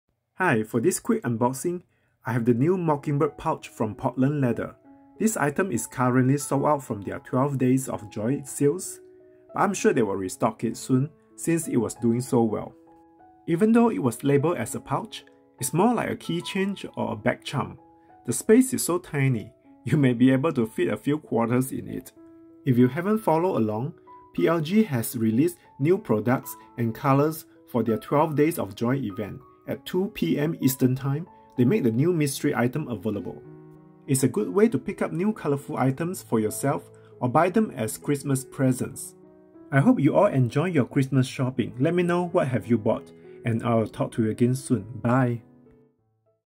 Portland Leather 12 Days of Joy Unboxing & First Impression - Mockingbird Pouch
I try my best to not purchase everything Portland Leather releases for their 12 Days of Joy event. I did pick up a few things and a few bags, lol. So, I guess more PLG unboxing videos are on the way. This Mockingbird pouch is cute but tiny. It is more like a keychain or a bag charm, so don't expect to put anything bigger than a quarter inside; it won't fit. It is super cute, though; I love it! Have you bought anything from 12 Days of Joy?